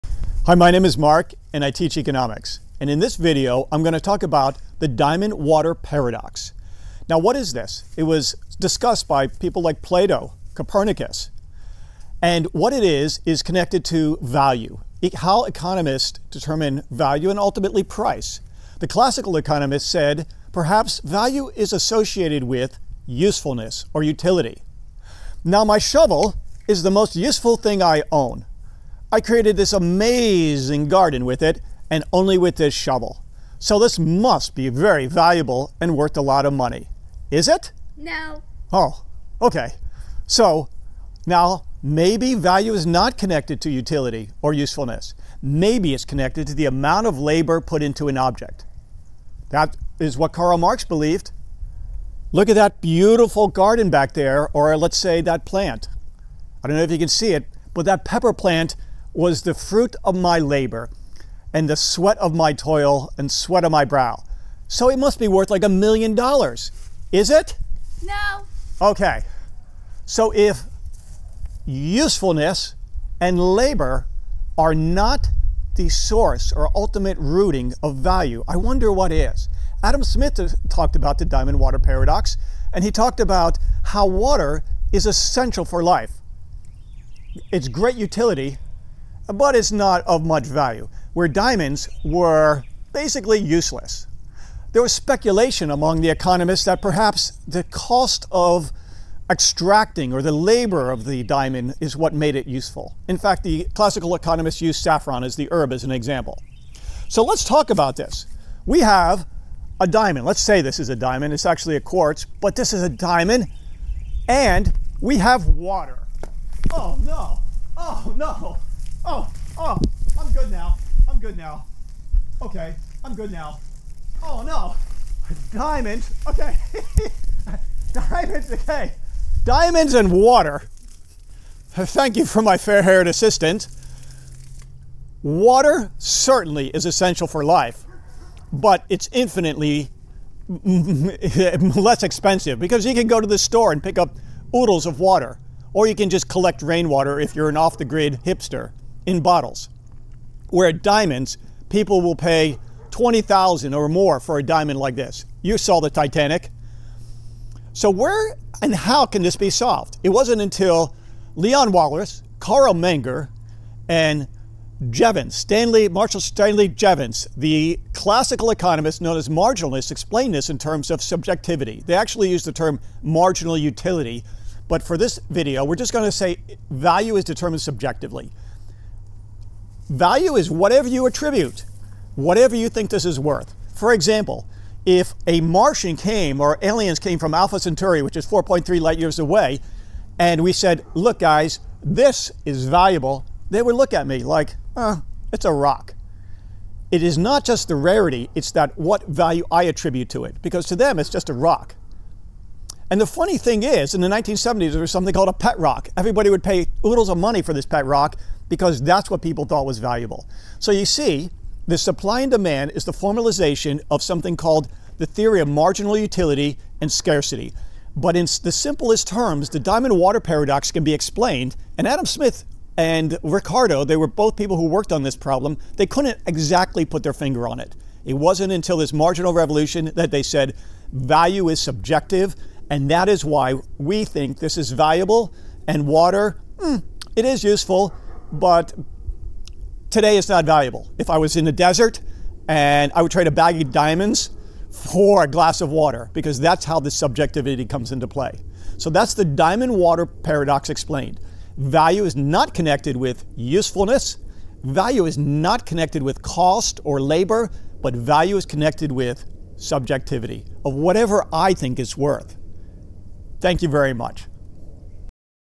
Diamond Water Paradox - This Explains It
The Paradox of Value was something which Adam Smith pondered. It might seem obvious today, but these bright minds of the classical era could not express exactly why Diamonds, something with little usefulness has great value, in contrast, water which is essential for life, is ‘cheap’. It was explained during the Marginal Revolution with the ideas of marginal utility and scarcity, which we derive supply and demand curves in a formalization. Expressed in simplest terms the solution is value is subjective General Utility, labor or cost does not play a role. This understanding rebutted Marx’s labor theory of value and explained why some things which are seemly useless, command such a high price.